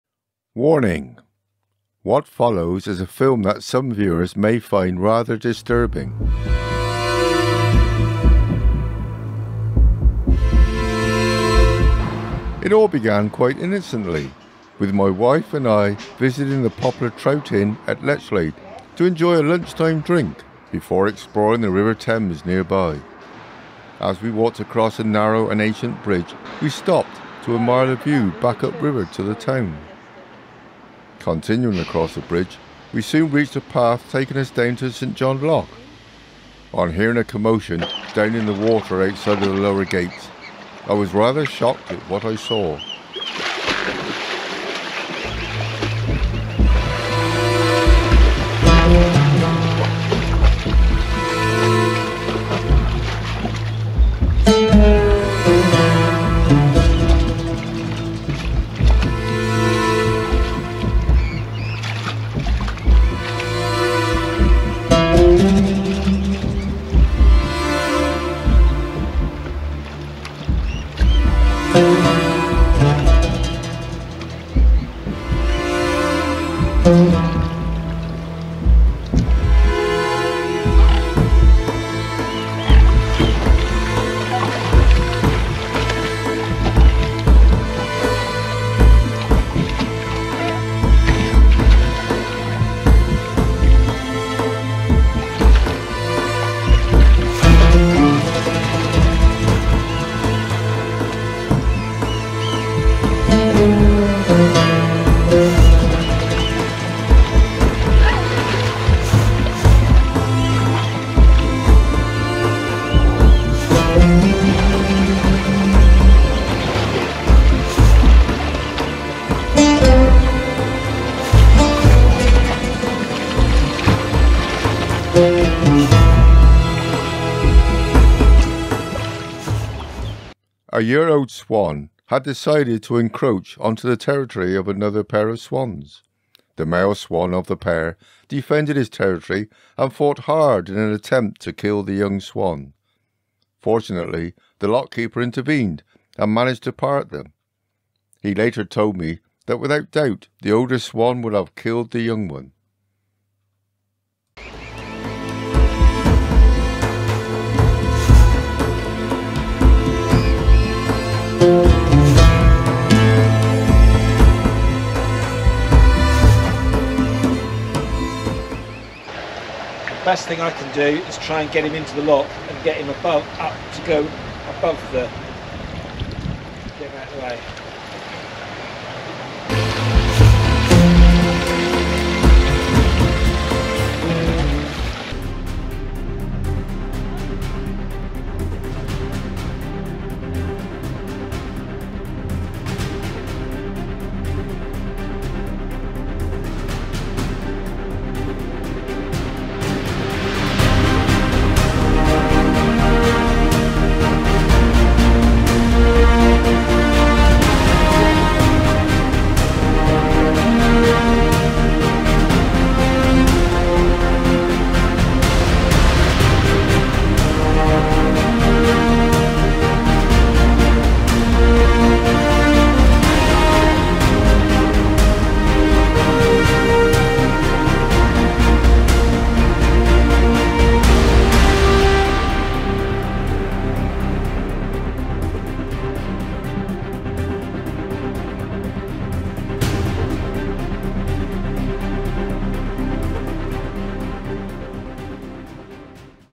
Witness to an Attempted Murder!
I filmed an attempted murder whilst out on a day visit to the Cotswolds. Some viewers may find this film disturbing to watch. An attempted murder is something you don’t wish to see.